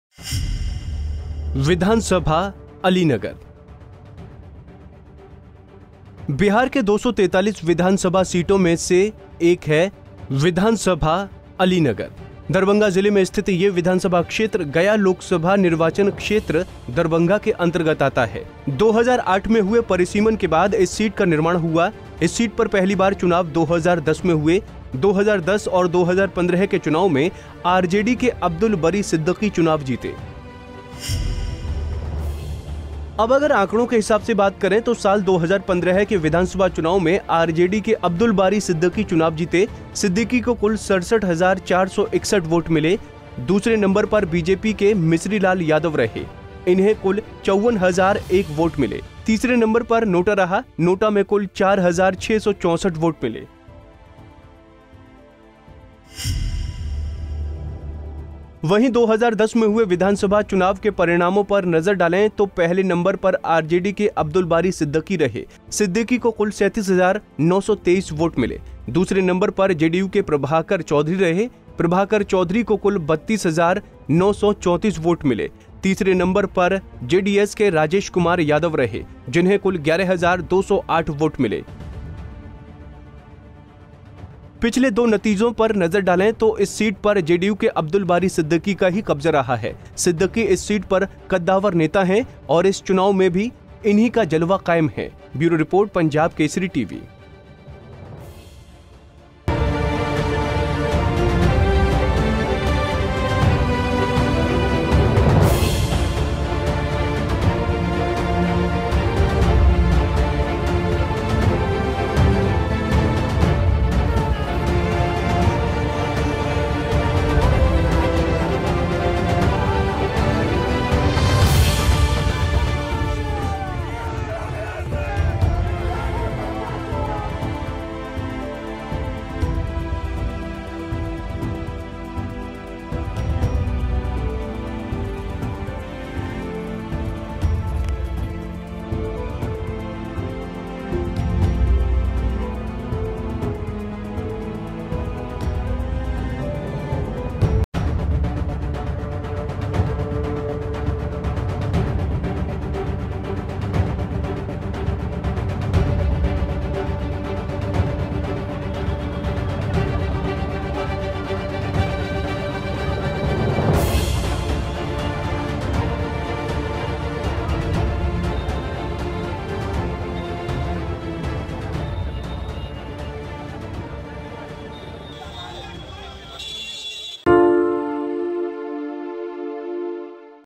Alinagar Assembly Seat II अलीनगर विधानसभा सीट के पिछले नतीजे II Alinagar
बिहार के 243 विधानसभा सीटों में से एक है बेनीपुर विधानसभा सीट है...दरभंगा जिले में स्थित यह विधानसभा क्षेत्र गया लोकसभा निर्वाचन क्षेत्र दरभंगा के अंतर्गत आता है... 2008 में हुए परिसीमन के बाद इस सीट का निर्माण हुआ...इस सीट पर पहली बार चुनाव 2010 में हुए, 2010 और 2015 के चुनाम में आरजेडी के अब्दुल बारी सिद्दीकी चुनाव जीते।